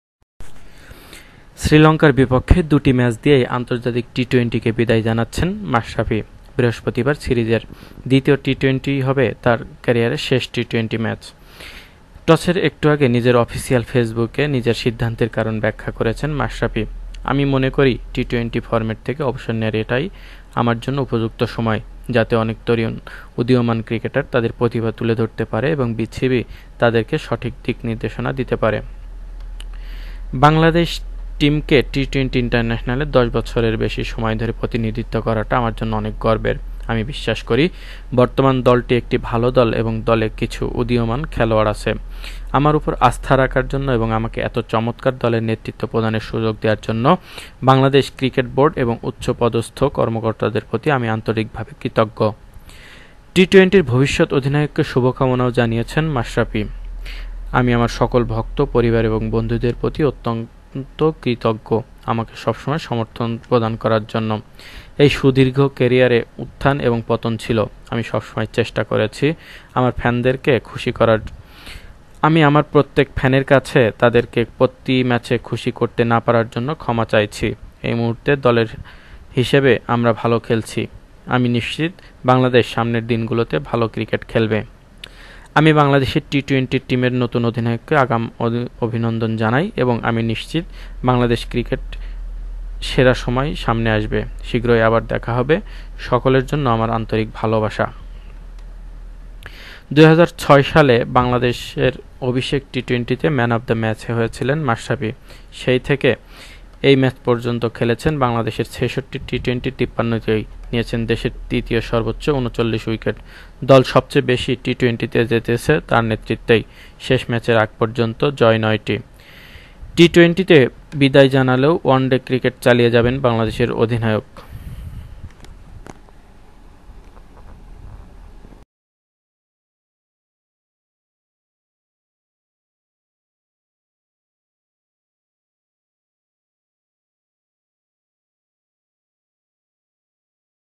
অবসর নিলেন মাশরাফি | Mashrafe to retire from T20
Mashrafe Mortaza has announced his retirement from T20 internationals during the toss of the first T20I between Sri Lanka and Bangladesh in Colombo. Mashrafe said that the current series would be his last in the format.